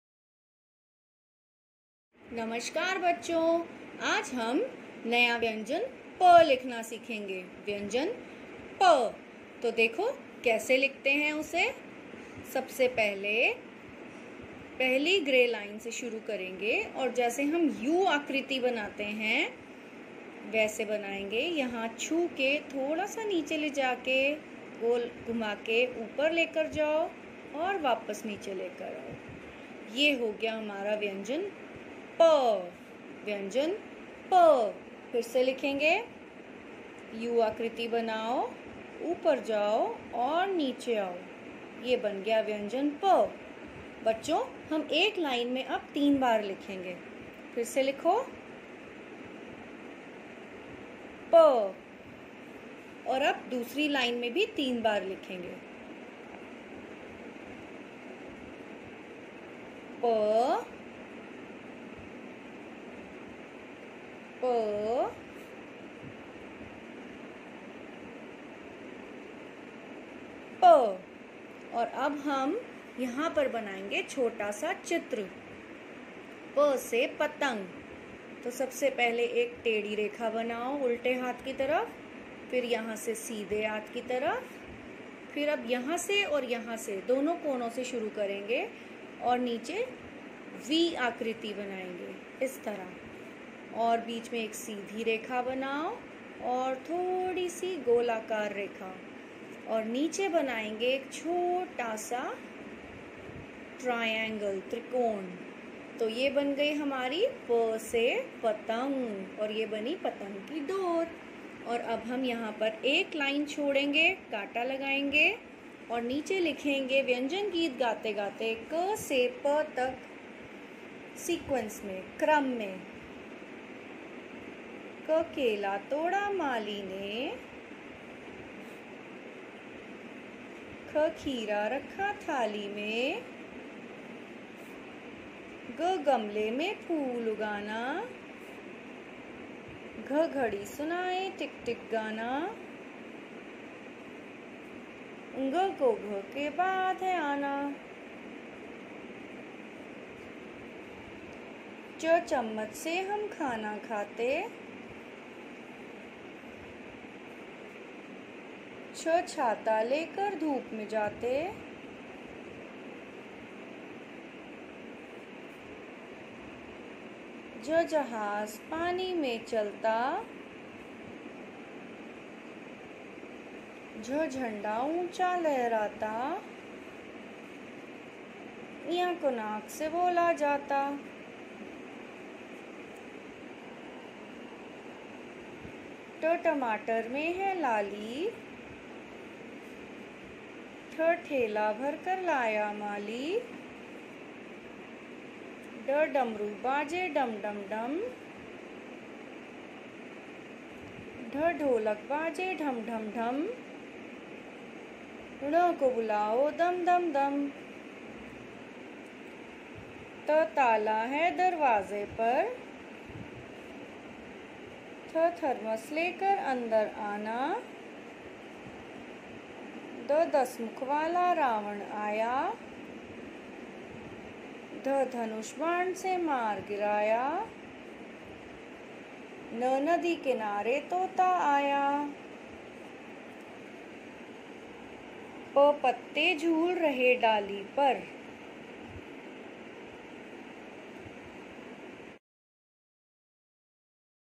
HINDI VYANJAN 21 'प' formation DLDAV MODEL SCHOOL, PITAMPURA
Greetings from Shimpi Wadhwa’s Classroom!!
This video will help you to make the children form the vyanjan correctly and write vyanjans क - प in sequence while singing vyanjan song along so that they don’t tend to forget the sequence of vyanjans.

Vyanjan Song 👇🏻

DLDAV Model School, Pitampura

Always looking forward to your valuable inputs & comments 🤗
Don’t forget to like 👍🏻

Shimpi Wadhwa 🌺🌺